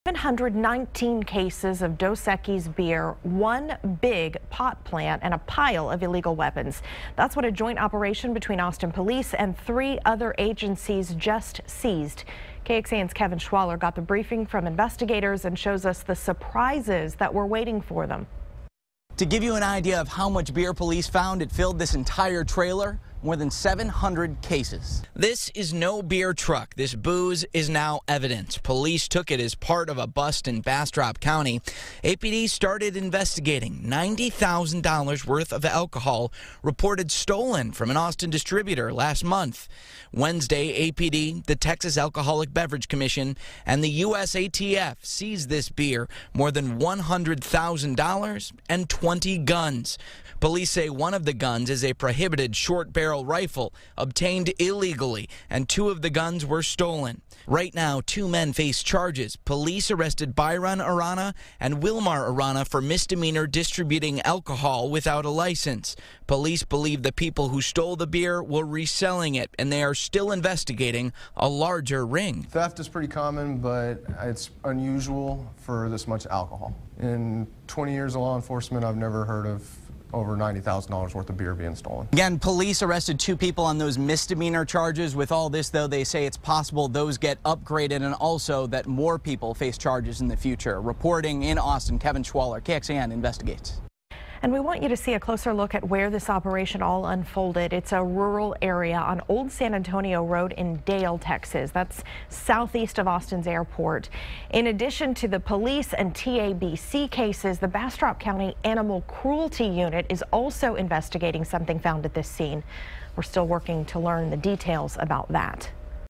719 cases of stolen beer, firearms seized from Bastrop County property
The investigation started in September when someone broke into a beverage distributing company and stole the alchohol.